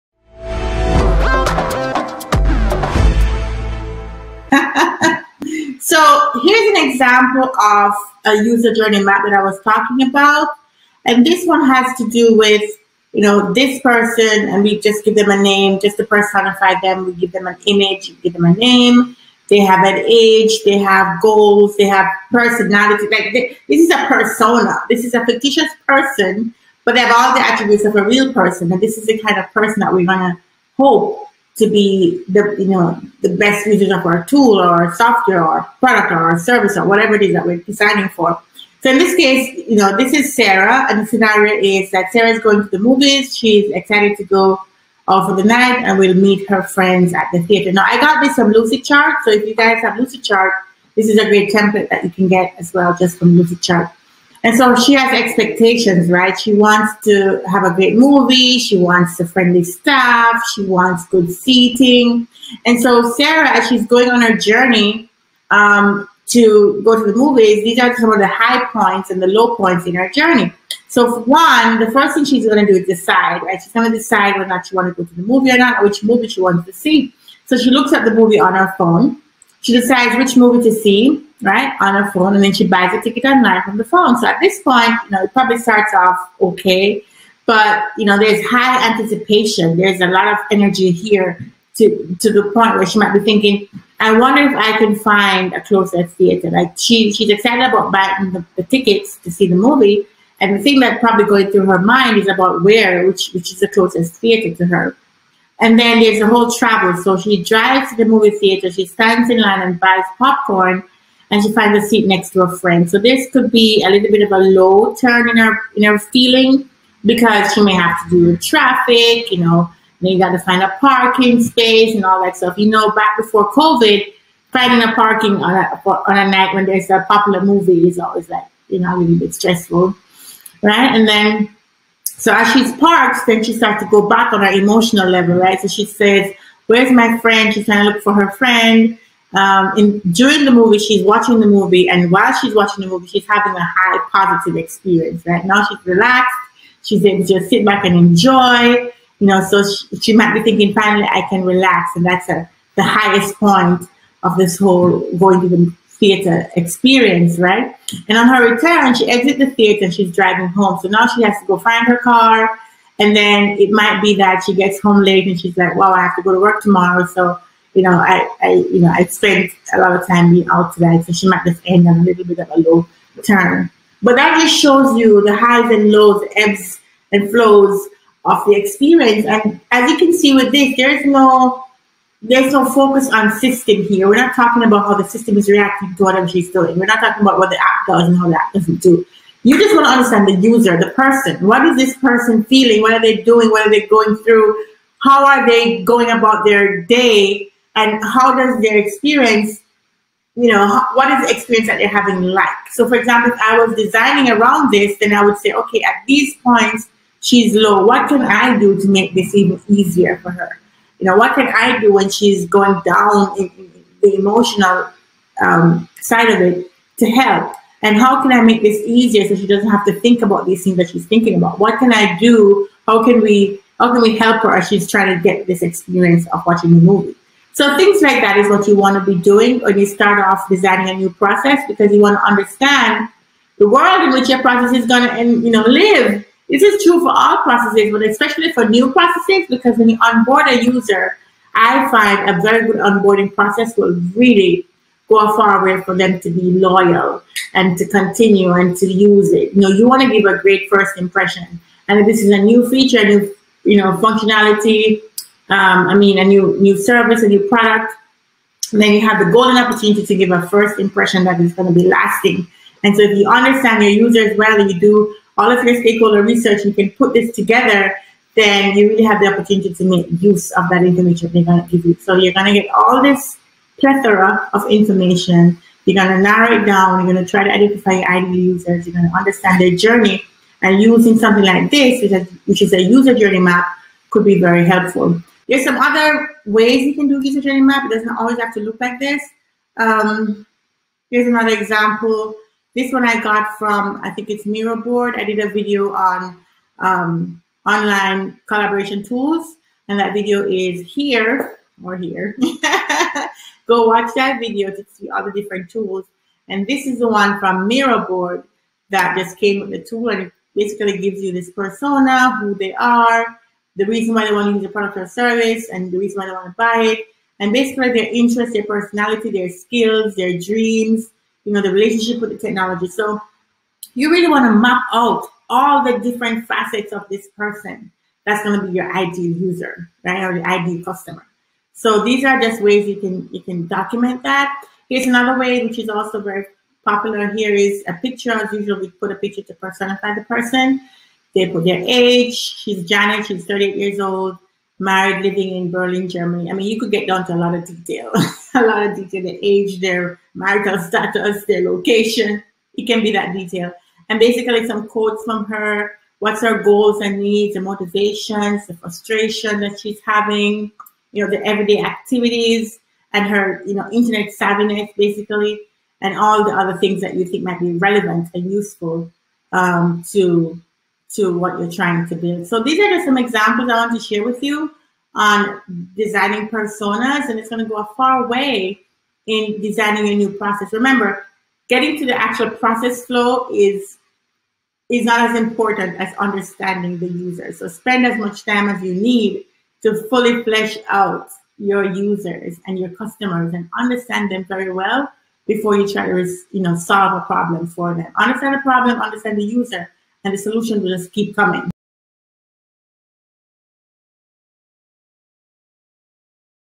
User Personas and User Journey Maps Examples
This video walks through an example of creating a user persona and user journey map. This is especially useful if you are creating a new process from scratch.

This video continues to give tips on how to a design a new process, a new feature or a new product offering. It teaches steps and what to think about as you take on this challenge.